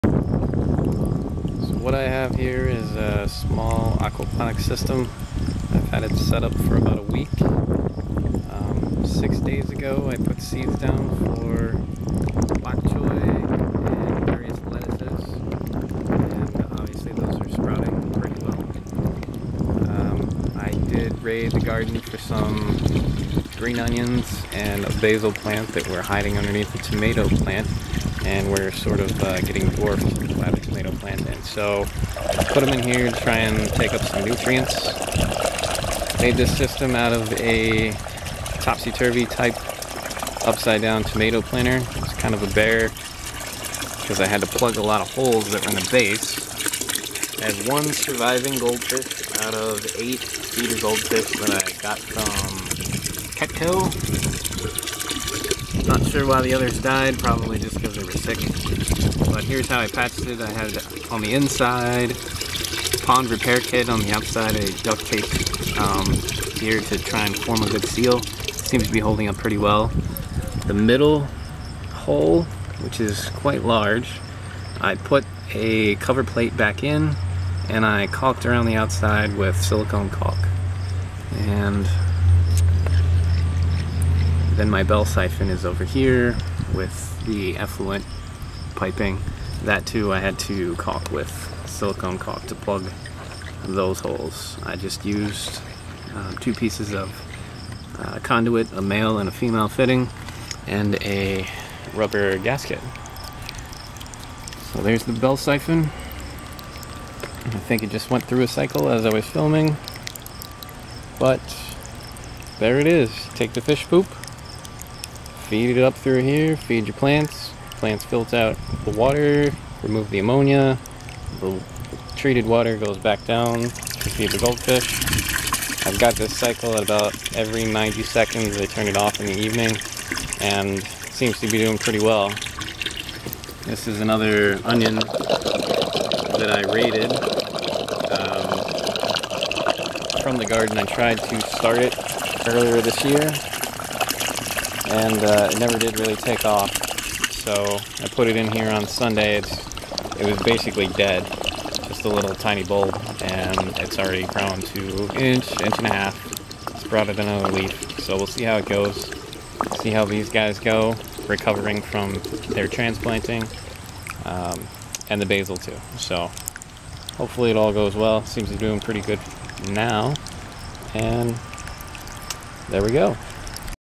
Small Aquaponics Setup - First Week
2 years ago, I bought a Suncast Tomato Planter from Blain's Farm & Fleet.  The frame itself is pretty sturdy but, being in a somewhat dry area and using the wrong kind of soil, my plants didn't do very well.  After learning about aquaponics, I wanted to try it out.

I covered all the plant holes with self-adhesive rubber from a pond liner kit and caulked the edges to prevent leaks.  I created a bell siphon with 2 inch conduit (male and female threaded pieces) and a 2 inch rubber gasket.  I caulked the piece inside of the basin and then added the remaining parts.  The aquarium portion is an 8 gallon plastic pot and I have a pond fountain pump cycling the water up to the growbed. Everything cycled for a few days before adding 8 feeder goldfish from Petco.  Seven have died, likely due to illness and the change in water temperature.

Tomato Planter